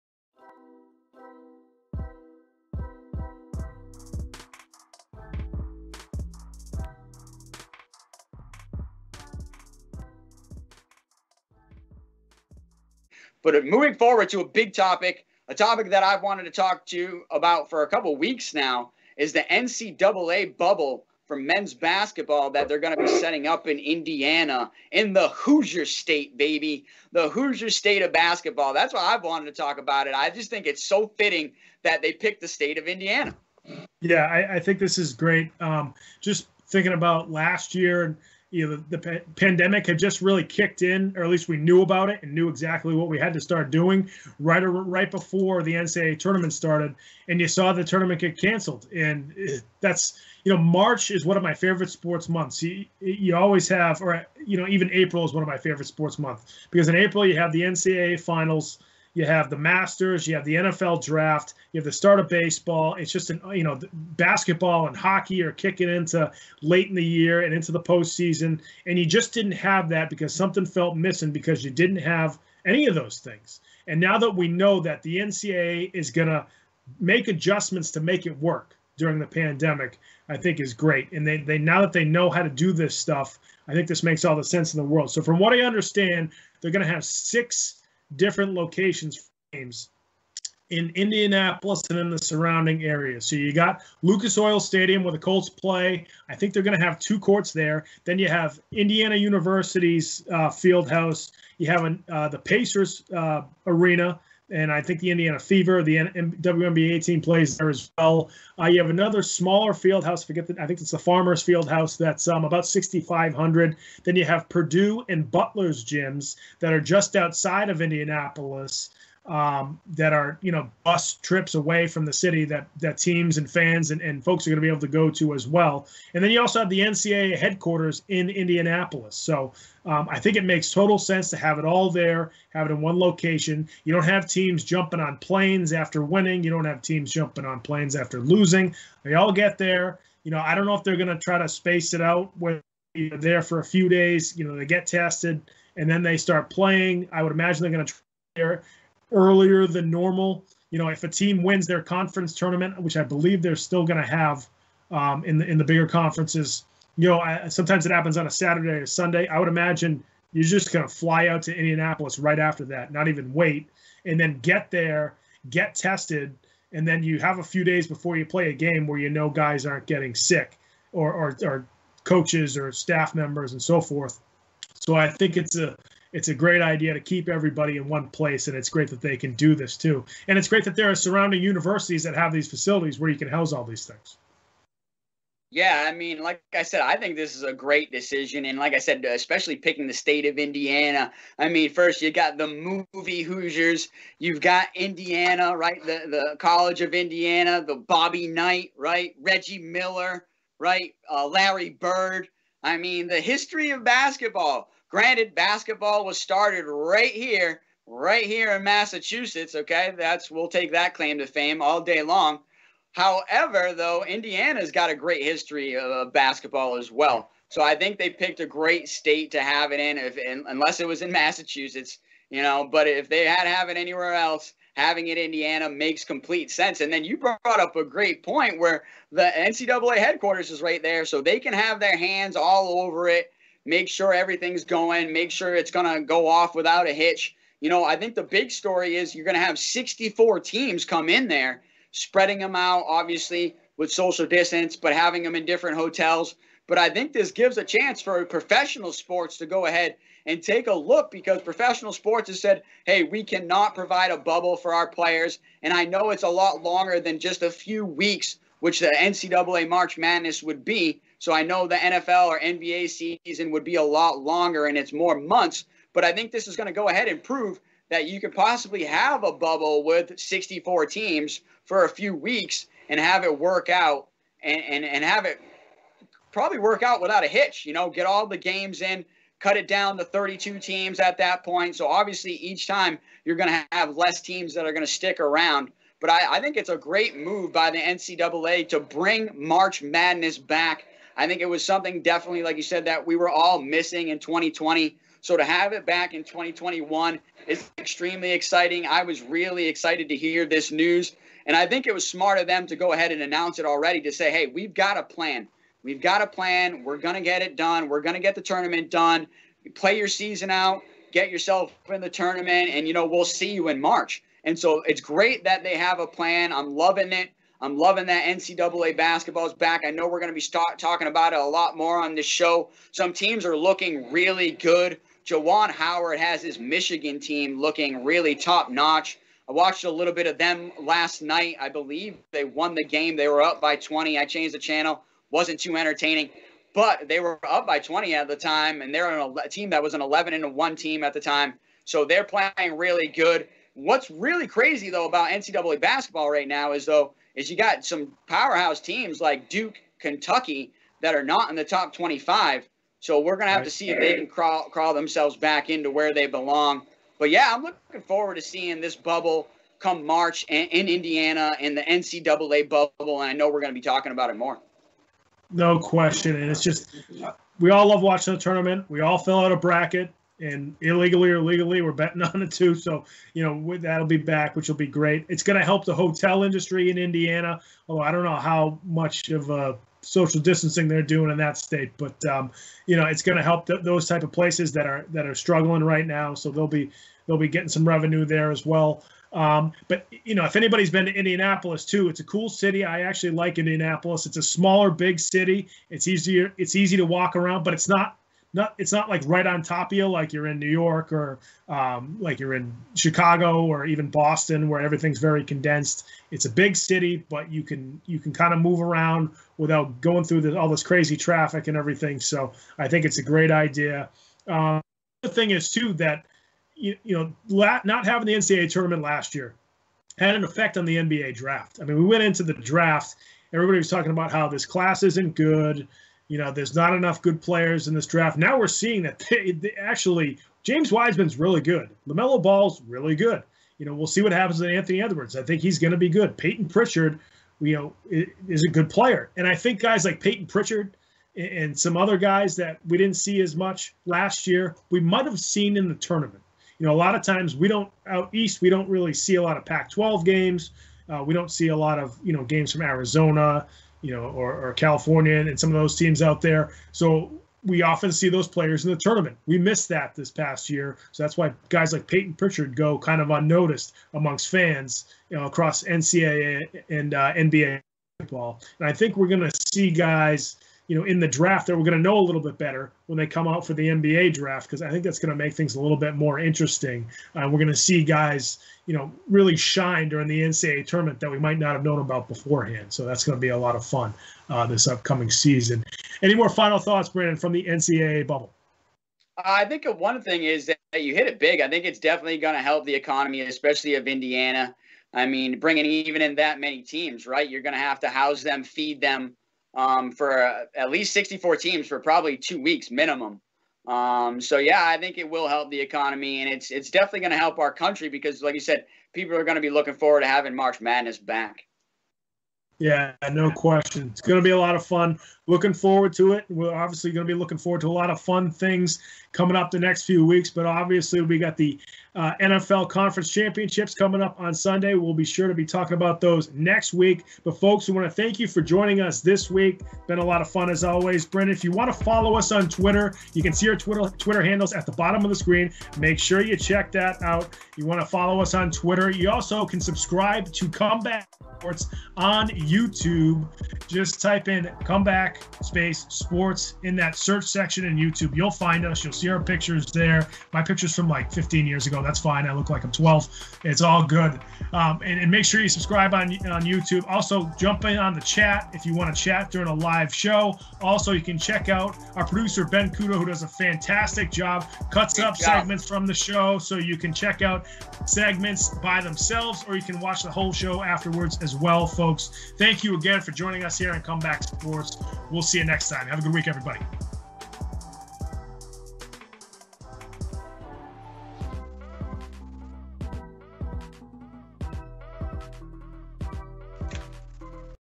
NCAA March Madness Bubble Indianapolis COMEBACK SPORTS Pat Shot Caller Flaherty Brandon Coach Perry
In-depth look at the world in sports. Weekly highlights, lowlights, and predictions. COMEBACK SPORTS!

1/20/21 - PAT AND BRANDON DISCUSS
10. NCAA BUBBLE MADNESS - NCAA Mens Basketball March Madness Tournament will take place in Indianapolis, IN. What to expect.